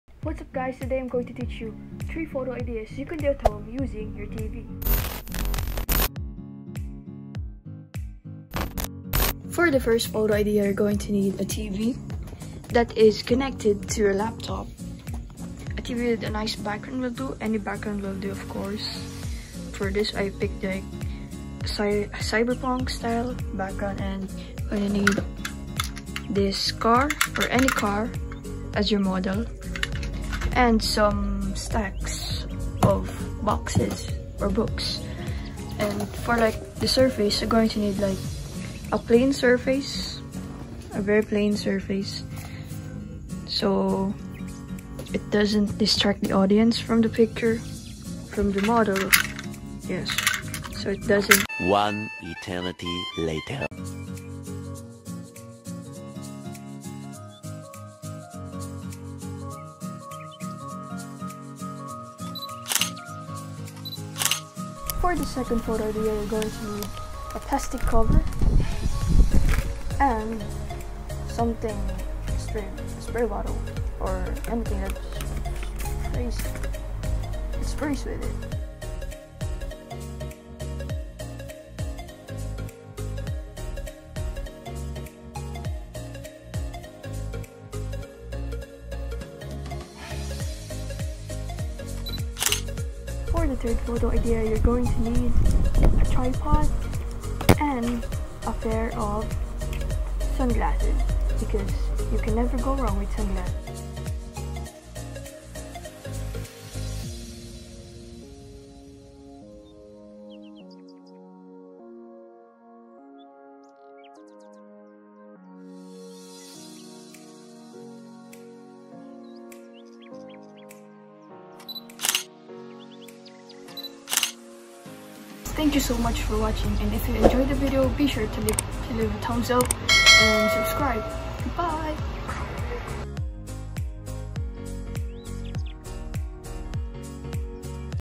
Photography Ideas at Home w/ Your TV! | Vlog #1
What's Up, Guys! I hope you're having a great day. Today, I made a video about the photo ideas I had at home using your tv connected to your laptop. Be sure to watch the whole video so you would not miss any part.

- credits to 7th Era for the second photo idea